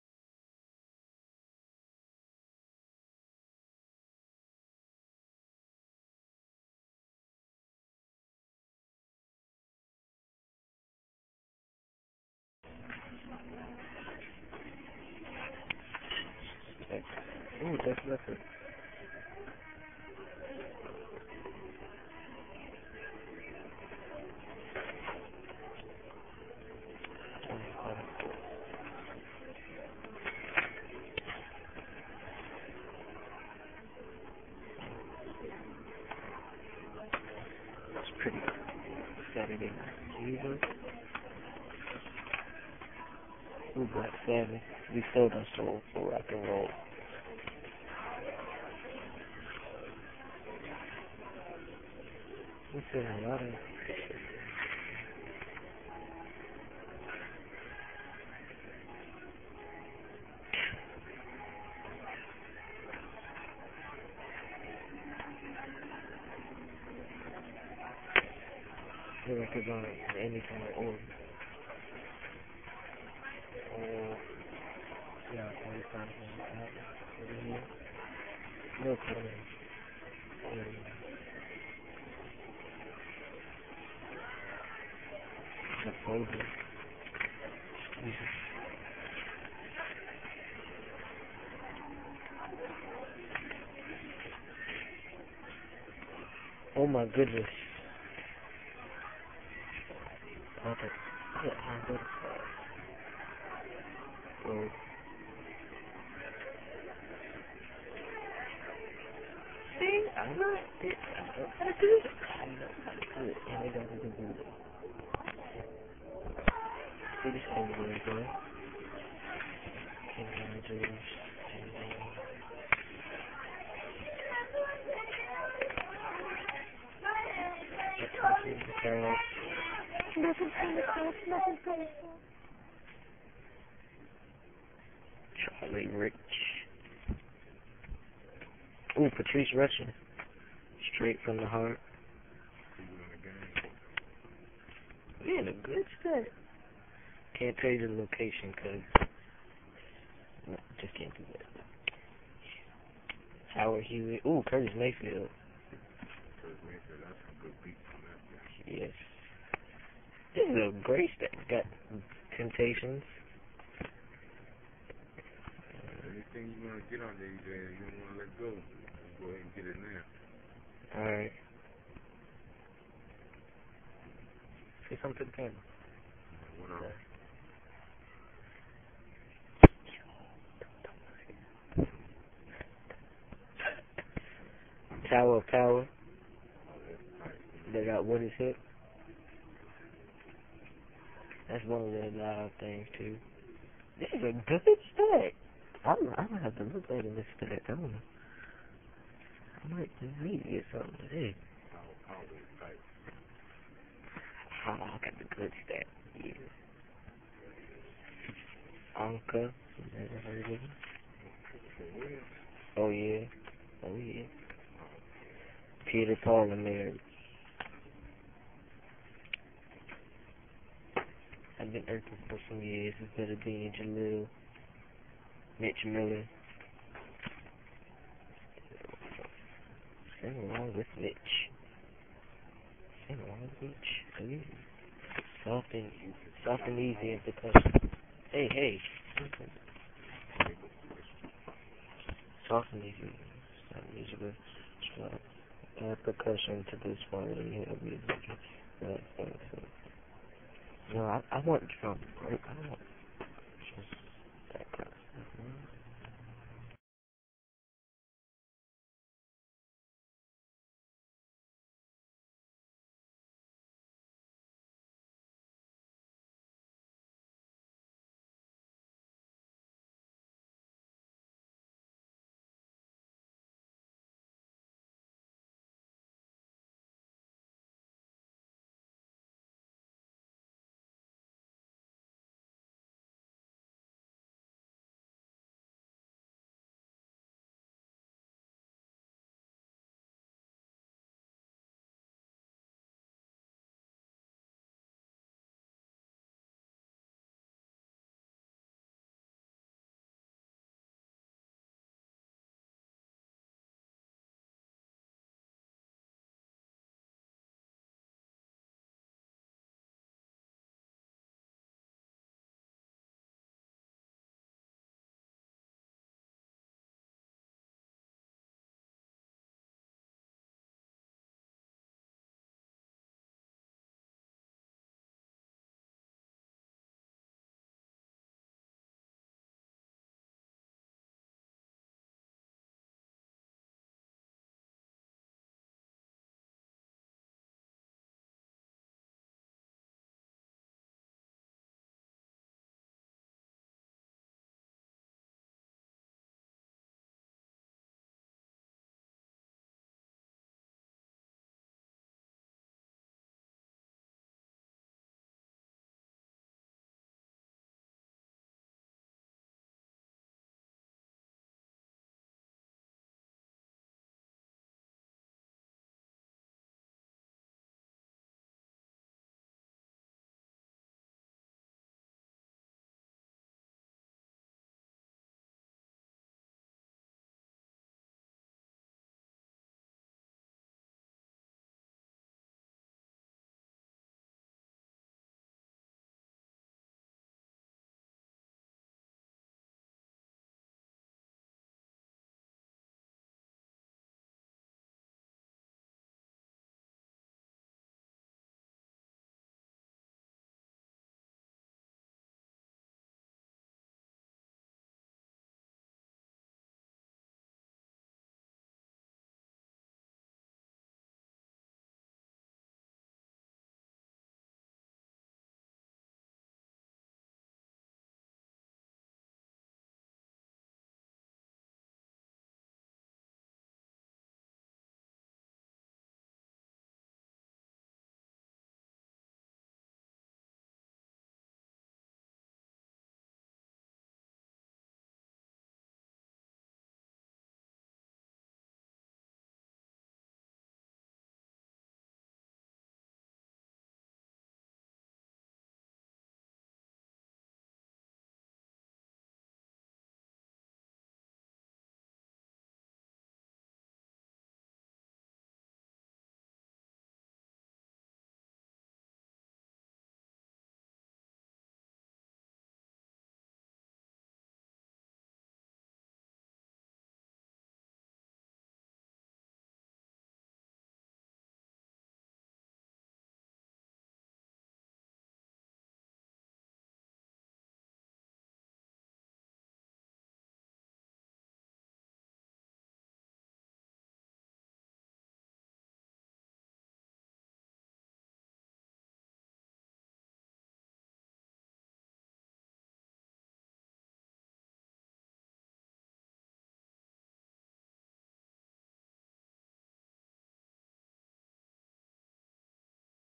Thrift Store Diggin'
Me and my dad in the Hampton Roads area of Virginia (My favorite place on earth) crate diggin' in thrift stores